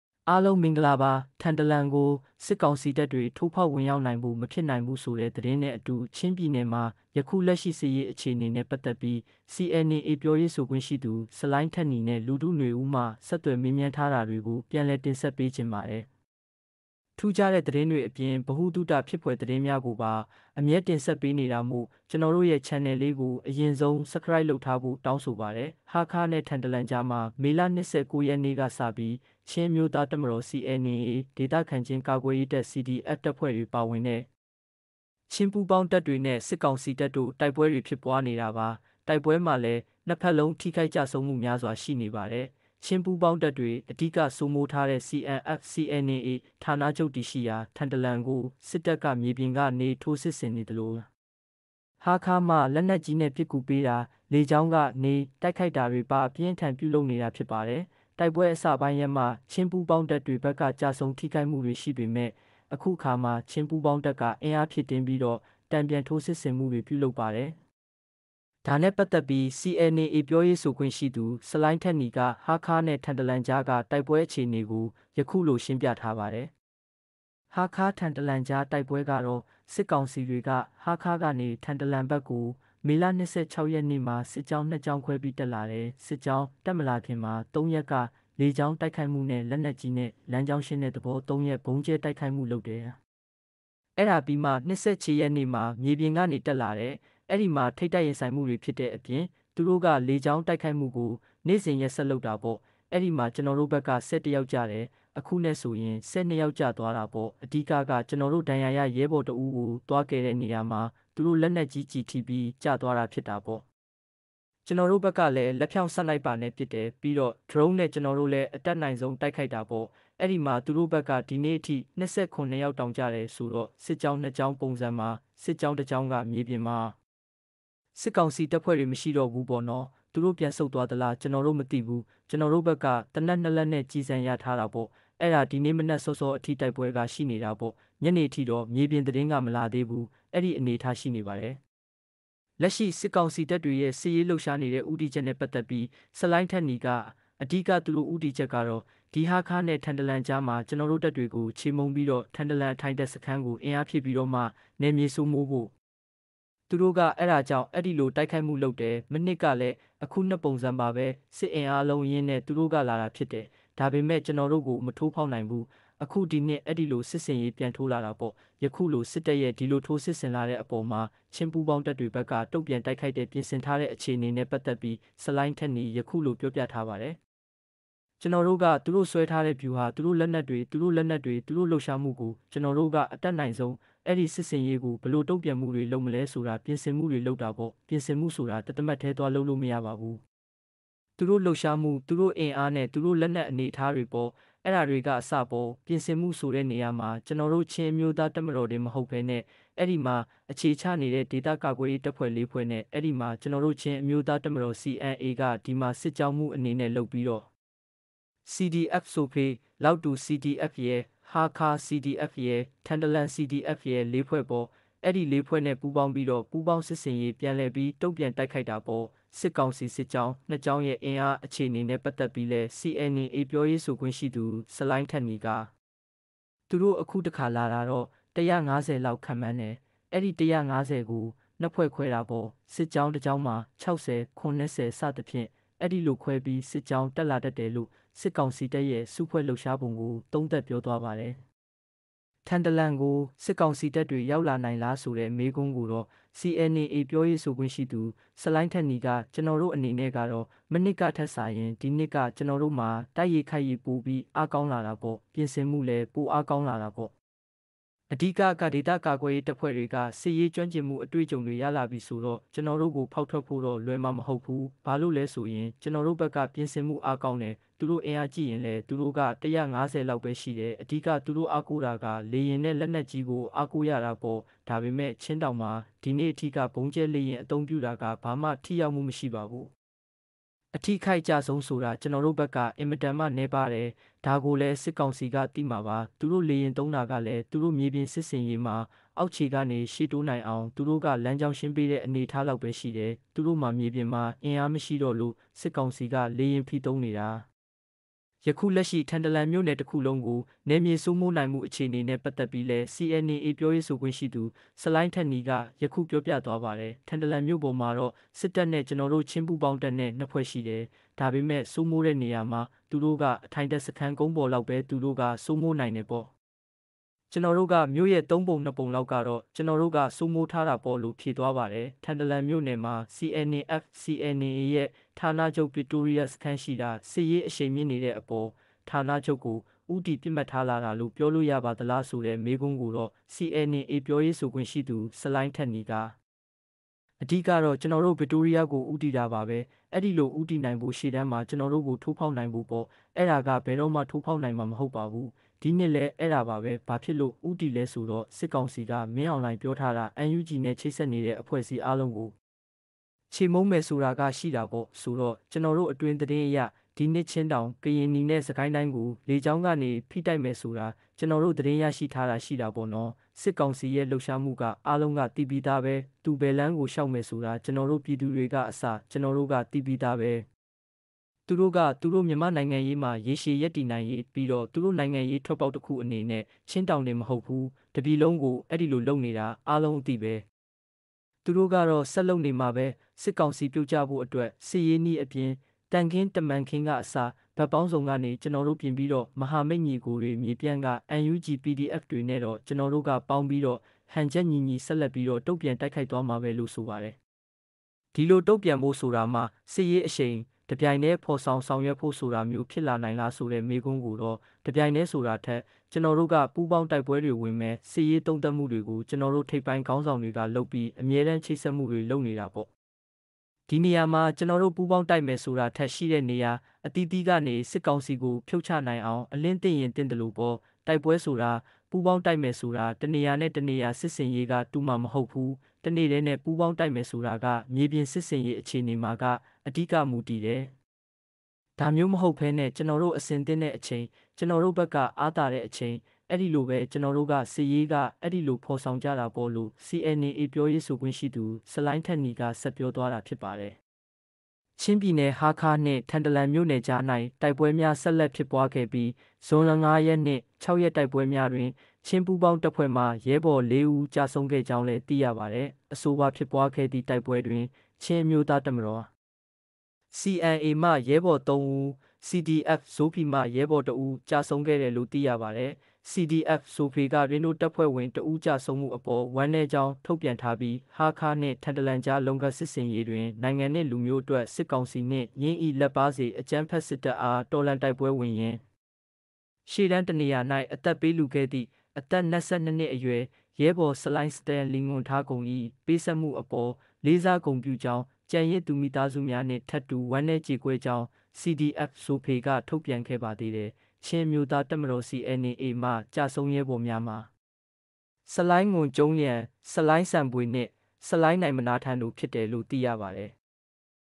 ထန်တလန်မြို့ကို စစ်ကောင်စီတပ်တွေ မရောက်နိုင်ဘူး
ချင်းအမျိုးသားတပ်မတော် ပြောရေးဆိုခွင့်ရှိသူ ဆလိုင်းထက်နီနဲ့ လူထုနွေဦးမှ တွေ့ဆုံ မေးမြန်းချက်ကို ပြန်လည်တင်ဆက်ထားပါတယ်